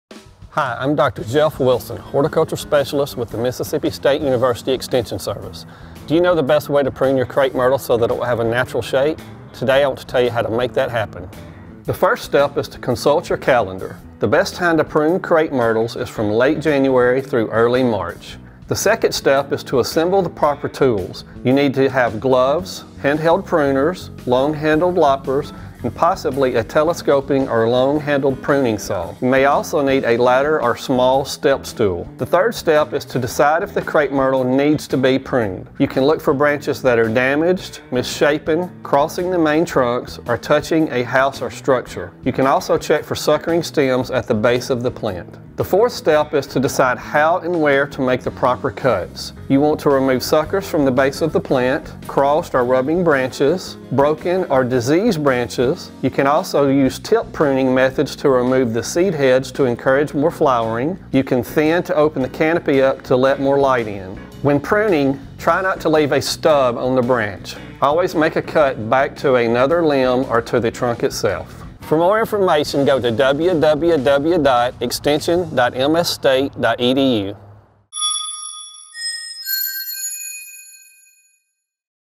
Crape Myrtle Pruning
Learn the best way to prune your crape myrtles! Dr. Jeff Wilson is a horticulture specialist with the MSU Extension Service.